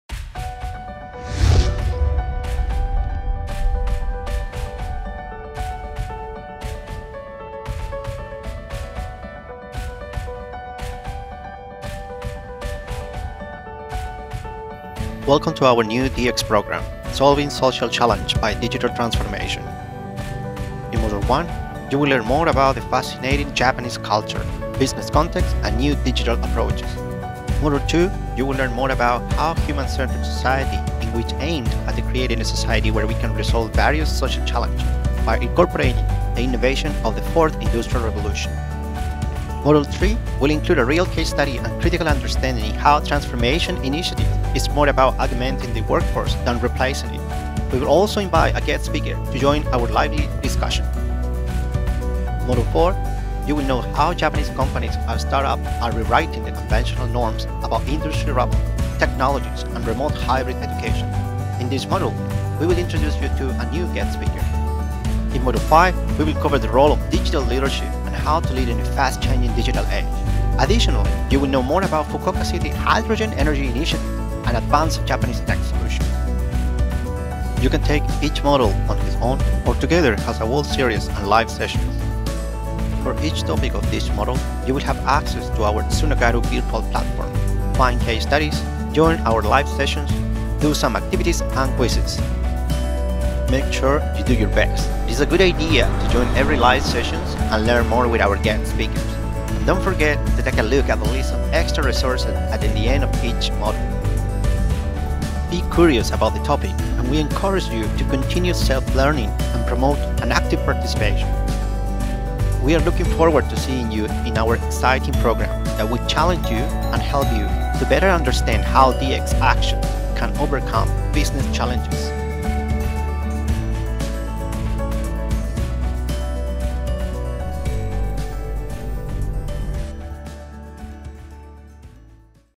Intro Online Course Solving Social Challenges by Digital Transformation (DX) - Launching Soon
We developed an introductory video about this new program to release focus on social challenges and how we can resolve with digital transformation (DX). The main idea of this program will be focus to identify key opportunities and potential to implement DX into any public and private sector (In Progress).

Explore our case website to know more about our global case studies.